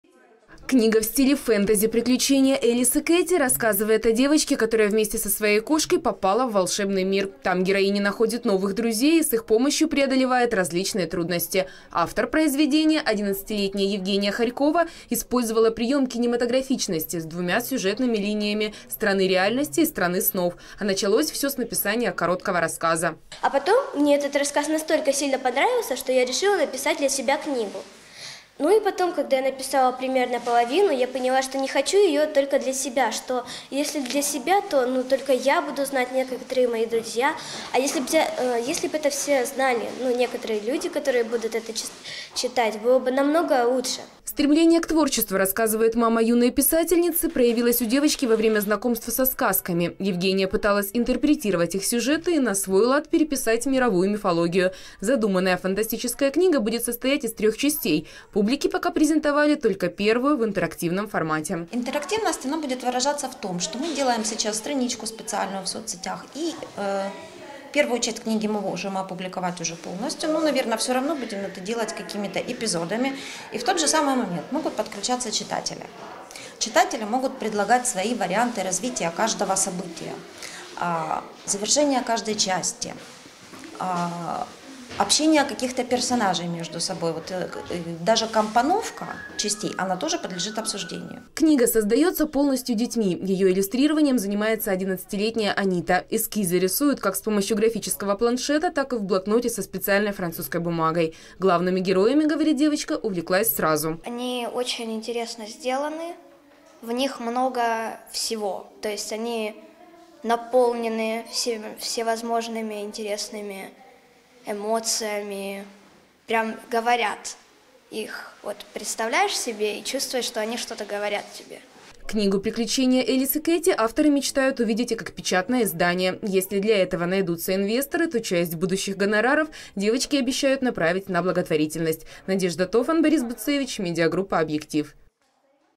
Юная харьковчанка написала книгу в стиле фэнтези
- Юная харьковчанка написала книгу в стиле фэнтези «Приключения Элис и Кэти» - Одиннадцатилетняя харьковчанка написала интерактивную книгу-игру для детей. Произведение планируется дополнить еще двумя частями, а стать их соавторами смогут все читатели